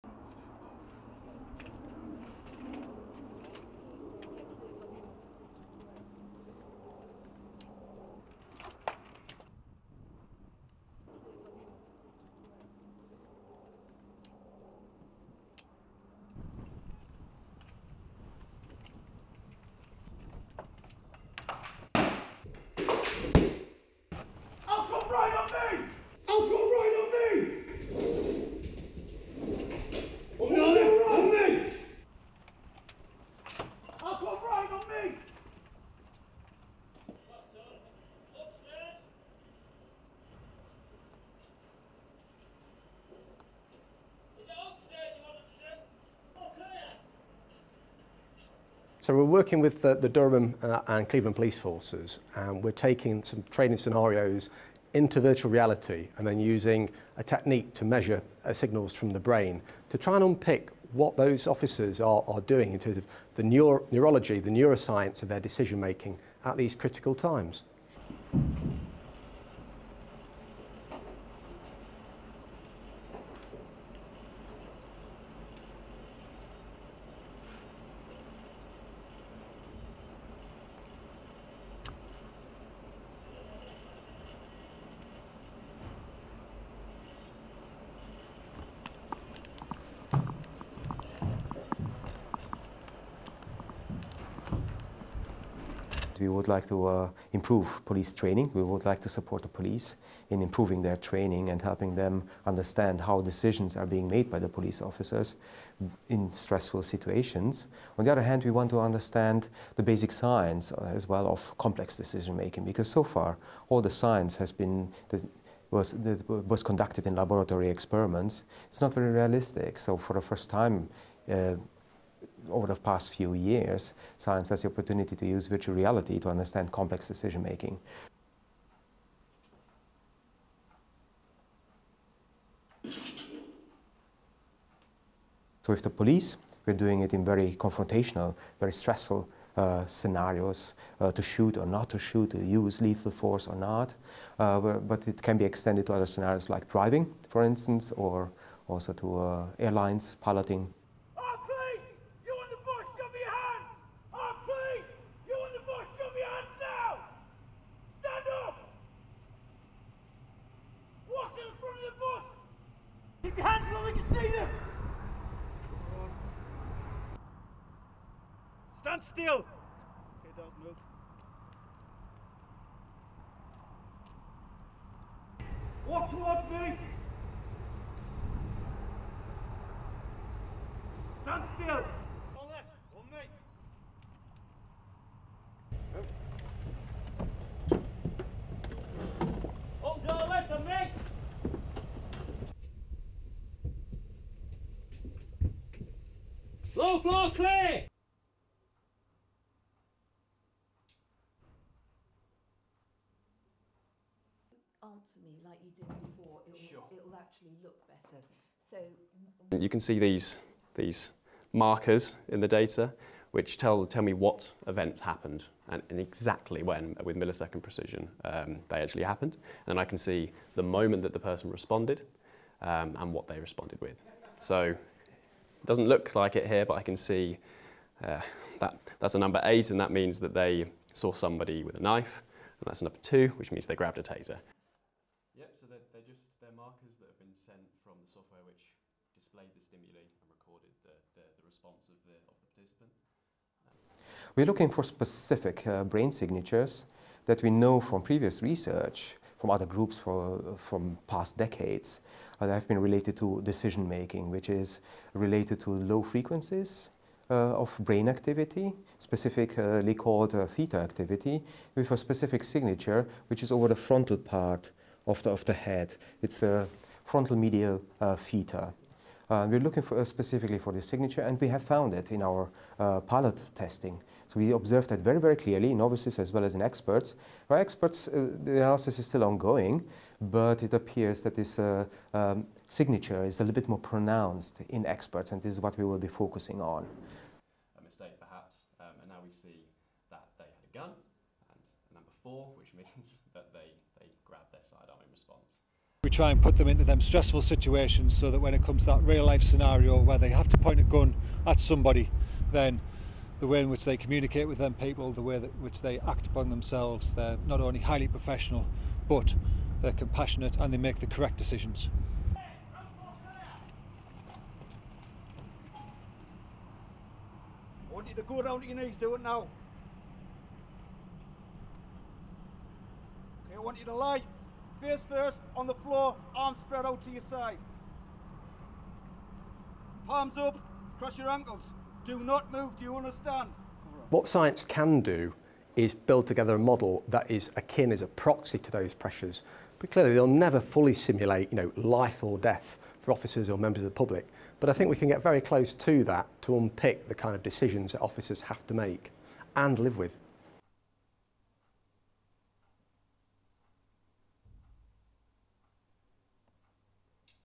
Scientists study police brainwaves for firearms suitability
(11 Apr 2019) LEADIN:
Scientists are studying the brain waves of trained firearms officers to work out how they respond in life and death situations.
They're using virtual reality to put officers through a number of different scenarios.
STORYLINE:
These are seasoned police officers, specially trained to use firearms.
Every year officers across the UK return to tactical training units like this one to ensure that police who bear guns can respond to dangerous situations calmly and safely.
Storming a building, or on the lookout for a sniper, the officers fall into their default response.
How to behave with armed suspects has drilled into these police teams in hour upon hour of training. Their response is almost automatic.
Police officers undergo an initial thirteen weeks firearms training here, learning not just how to shoot a weapon, but all the tactics involved with firearms training.
Now a pilot study is underway to see if scientists can identify differences in the brain activity of these experienced firearms officers, and people who have no training.
Former police inspector Shaun Beebe is now working with scientists at Nottingham University's School of Physics and Astronomy.
He says: "So we're working with the Durham and Cleveland police forces and we're taking some training scenarios into virtual reality and then using a technique to measure signals from the brain to try and unpick what those officers are doing in terms of the neurology the neuroscience of our decision making at these critical times."
The researchers have created a virtual reality situation where police officers are confronted with an avatar which appears from around a brick wall.
Randomly the avatar may be armed with a knife, a gun, or they may be completely unarmed.
The officer taking part in the experiment has to decide whether it is right to deploy a gun, or shock the suspect with an electric taser gun.
Over fifty minutes the officer in the experiment will face 120 snap decisions on whether to use his gun.
During the evaluation they wear an EEG (electroencephalogram) cap which measures and records their brain activity.
At the end of the session researchers can look at the EEG recording and see how an officer responded to a specific situation.
The lead researcher is Klaus Kessler Professor of Cognitive Neuroscience at Aston University.
He says: "We would like to improve police training. We would like to support the police in improving their training and helping them understand how decisions are being made by the police officers in stressful situations. On the other hand we want to understand the basic science as well, of complex decision making because so far all the science has been conducted in laboratory experiments. It's not very realistic. So for the first time over the past few years science has had the opportunity to use virtual reality to understand complex decision making."
Understanding the complex decision making that's going on here is not a easy task.
The scientists are looking at theta brainwaves.
Kessler and his team are at the very start of a long process, but they hope that in the future they will be able to identify people who have the best responses to risky jobs.
"So with the police we're doing it in very confrontational, very stressful scenarios, to shoot, or not to shoot. To use lethal force or not. But it can be extended to other scenarios like driving for instance, or also to airlines piloting," says Kessler.
Here officers are in a scenario where an armed suspect has taken a bus.
The software for the pilot study was designed by Nick Alexander from Aston University.